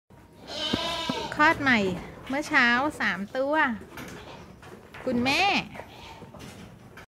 คุณแม่คลอดใหม่ เวทเนิส ม.มหานคร EP:783
มาถึงคอกแพะ ได้ทราบข่าวว่ามีแพะเกิดใหม่ 3 ตัว